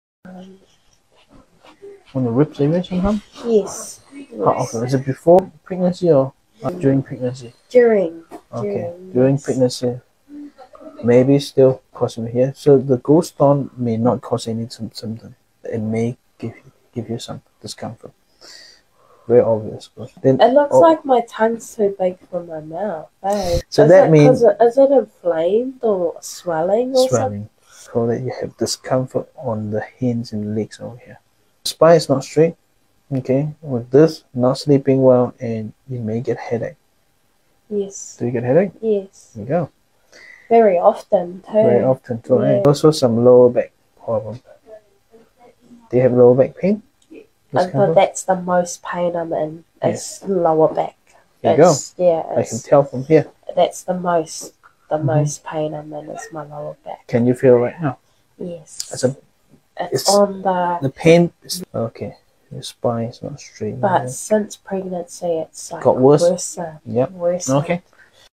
Your back pain has a story, and your tongue tells it without a whisper
Your back pain has a story, and your tongue tells it without a whisper. Before you can share your history of discomfort, a skilled TCM practitioner has already unveiled the tale of tension you carry. With a single glance at your tongue—its patterns, hues, and coatings—they unravel not just the overt symptoms of back pain, but also the subtle intricacies of your internal health. Patients are left in wonder at how such an unassuming yet profound diagnostic method can reveal so much, urging a deeper understanding and appreciation for TCM’s tongue diagnosis. It‘s an ancient art perfected over millennia—a testament to the holistic approach that views the body as an interconnected landscape. Explore how tongue diagnosis is an indispensable tool in TCM, bridging ancient wisdom with modern needs. It invites both patients and practitioners alike to ’speak‘ in a shared language of healing that predates words and brings to light the intricate narrative of our health.